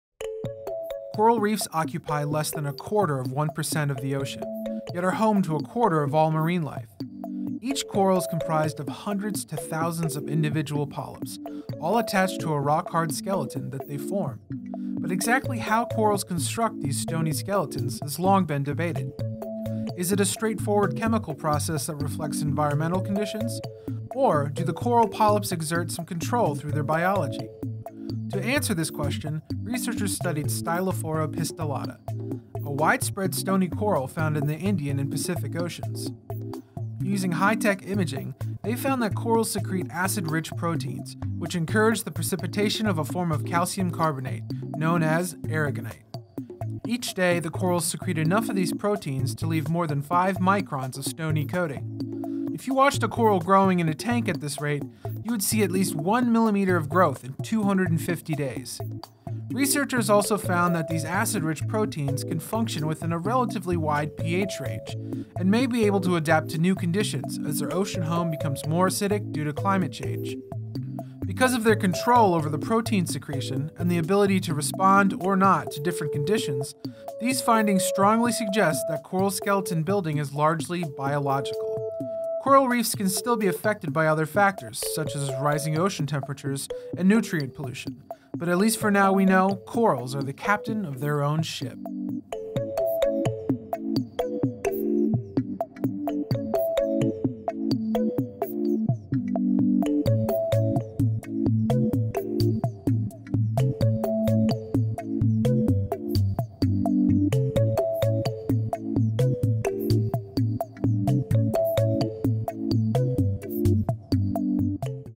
Corals can still grow their ‘bones’ in acid waters
New finding reveals how corals build their skeletons. Read the research ($)